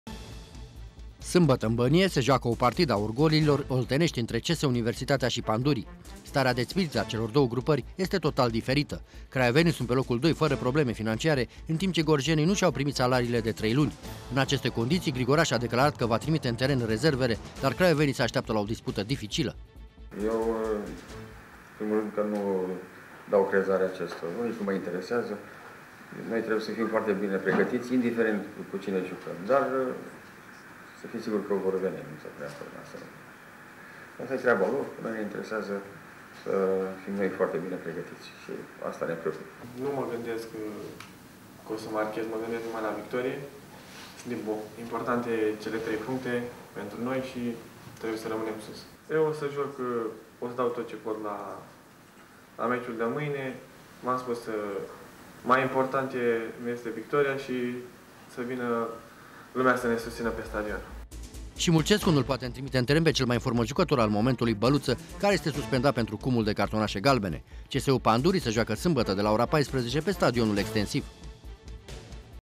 MERG LA VICTORIE
CSU joacă sâmbătă pe teren propriu în derby-ul Olteniei cu Pandurii. Gorjenii au probleme financiare şi au anunţat că vor trimite în teren echipa a II-a. Cu toate acestea craiovenii sunt pregătiţi să dea piept cu cea mai bună “garnitură” a lui Grigoraş. La finele turului gruparea din Bănie se află pe locul doi la trei puncte în spatele Stelei.